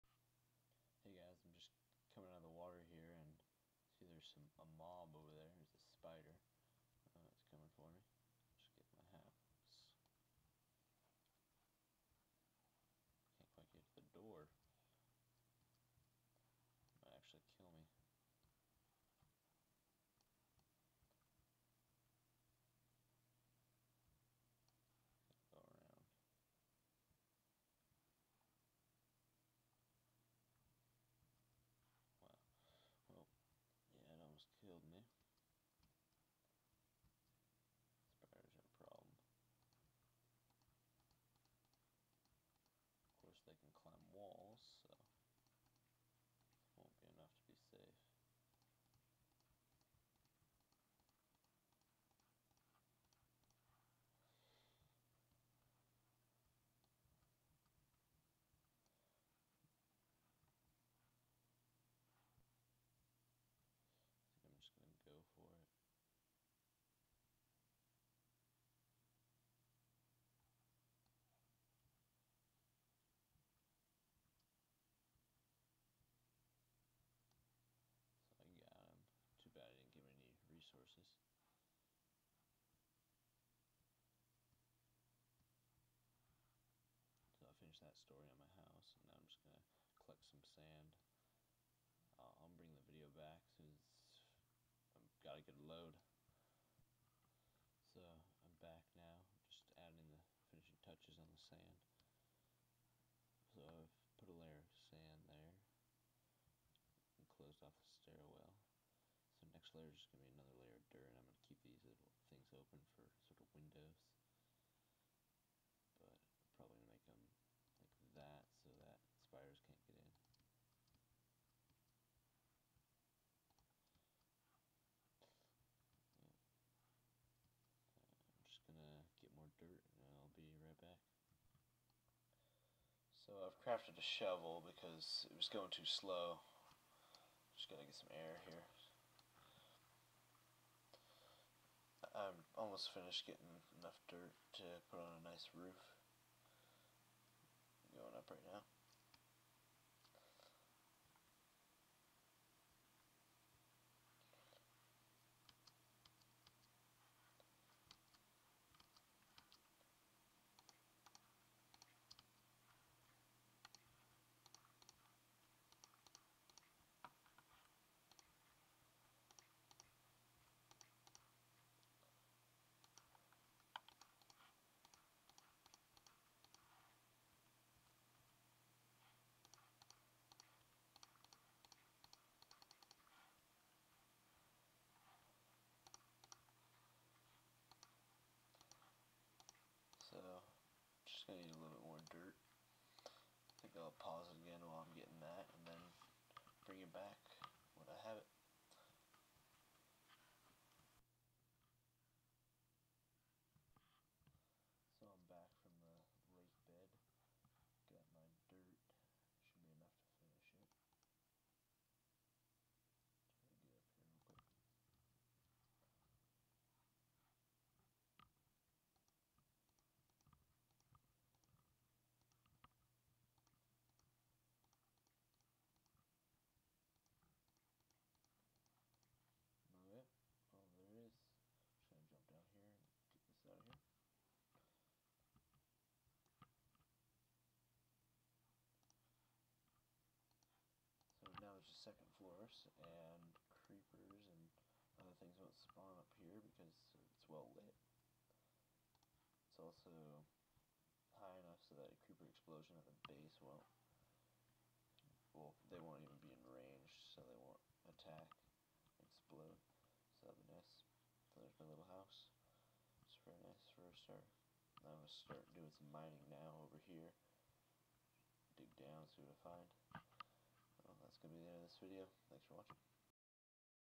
Survival Island - Part 2 - First Encounters
In this video I start to develop my house a bit and run into a spider, (sorry for terrible fighting I needed a sword). I now have a good house to start to develop my island from.